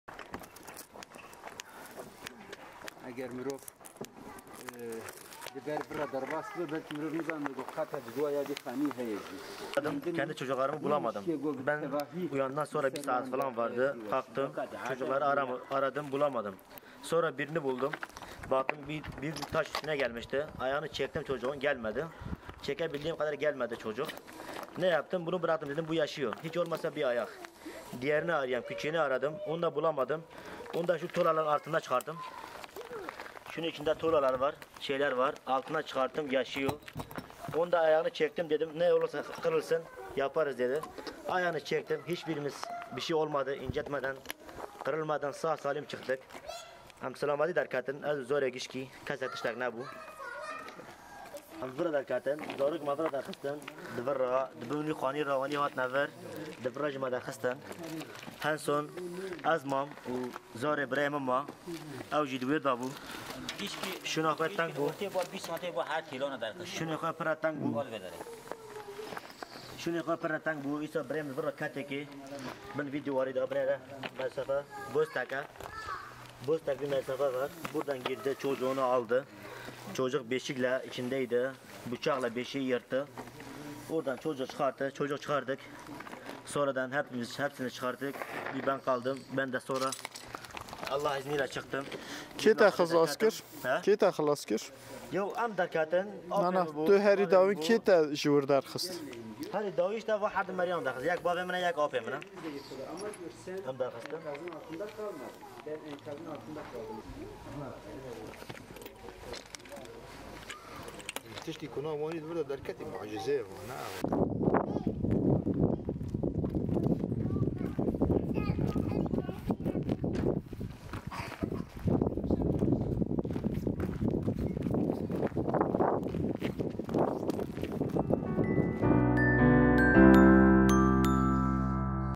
ENKAZ ALTINDA SOĞUKKANLI ÇABA: TÜM AİLESİNİ KURTARDI
Dulkadiroğlu ilçesinin Karahasan Mahallesi'nde Tarık Şex Mihemed adlı yurttaş, yerle bir olan evinin enkazının altında elleriyle açtığı yaşam koridorundan, ailesinden 6 kişiyi kurtardı.

Depremin merkez üssü Mereş’e bağlı Dulkadiroğlu ilçesinin kırsal mahallelerinde yüzlerce ev yerle bir olurken, birçok evde de ağır hasar oluştu. Deprem nedeniyle Karahasan (Doğanlıkarasan) Mahallesi'nde de birçok ev ağır hasar gördü. 7 obası bulunan mahallede, ağır hasarlı evlere rağmen can kaybı yaşanmadı. Enkaz altında kalan birçok kişi, yakınları ve komşuları tarafından kurtarıldı